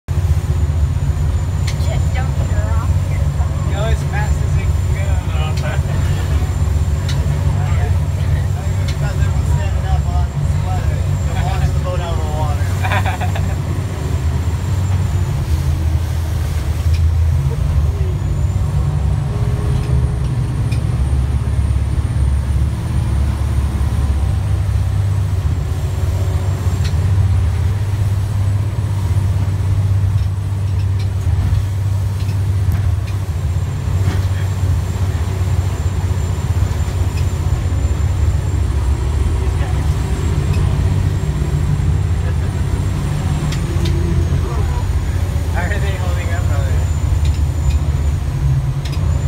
Boat ride with the US Coast Guard Kauai.